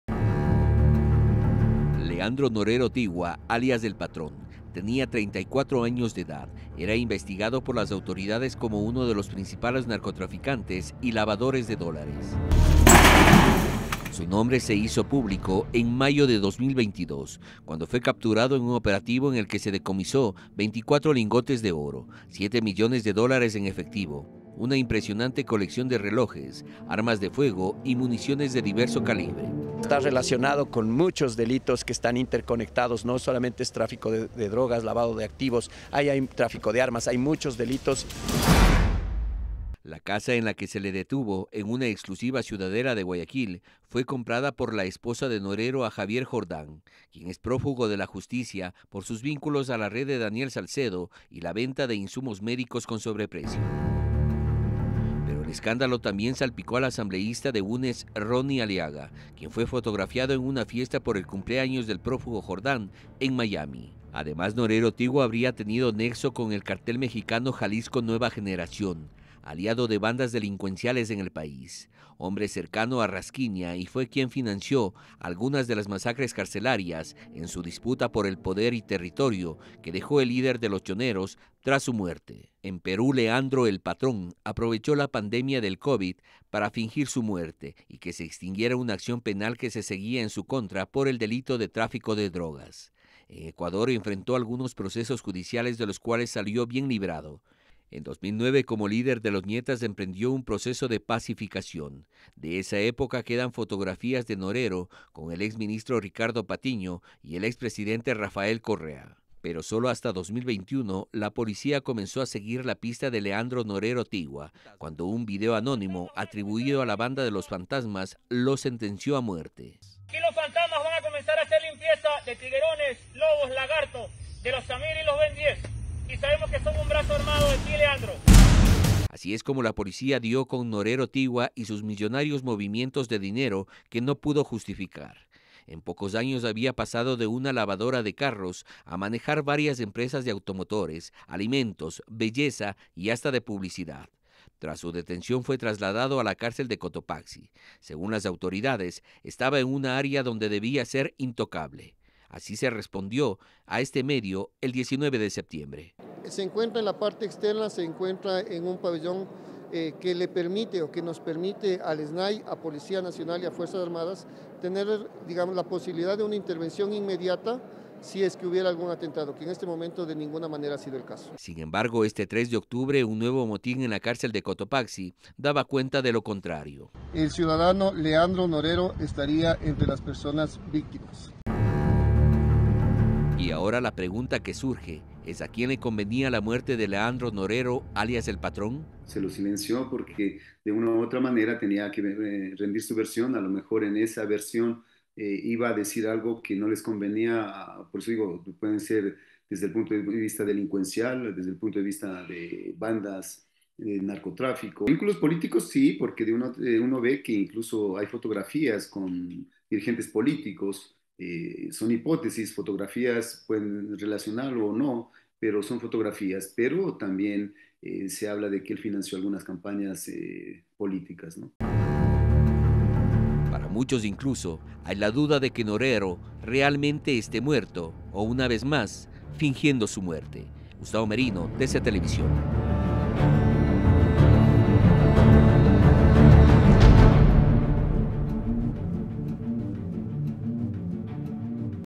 QUIEN FUE LEANDRO NORERO TIGUA ALIAS ¨EL PATRON¨
Las autoridades han confirmado la muerte de Leandro Norero Tigua en el motín de la cárcel de Cotopaxi este lunes. Pero quienes este presunto narcotráficante, alias ´El Patrón´  ¿Cuáles eran sus vínculos y por qué lo querían Muerto?

Patricio Carillo - ex Ministro del Interior ( archivo)

Edison Romo - Analista en Seguridad